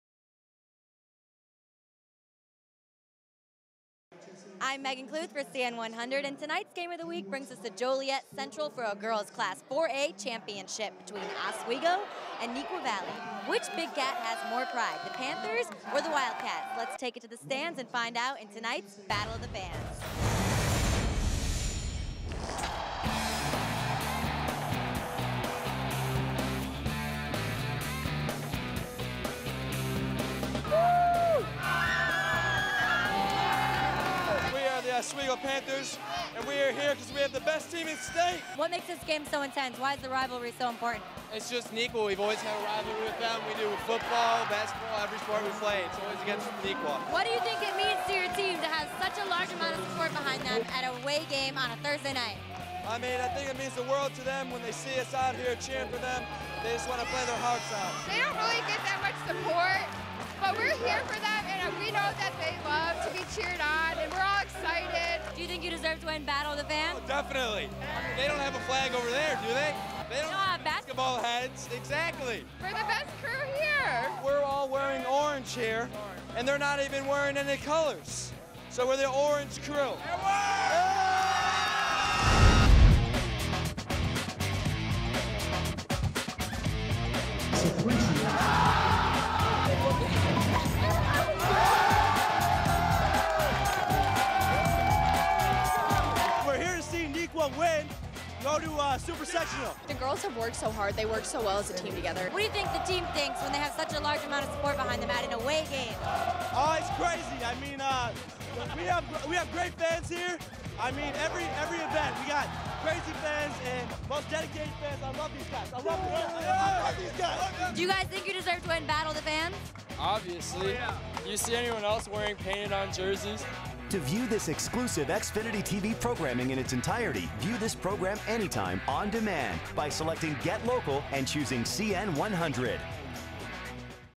Oswego Panthers vs. Neuqua Valley Wildcats
Join the Game of the Week as we take a look at the fans of the Oswego Panthers vs. Neuqua Valley Wildcats - Boys Basketball - Season 5 of Battle of the Fans 2014.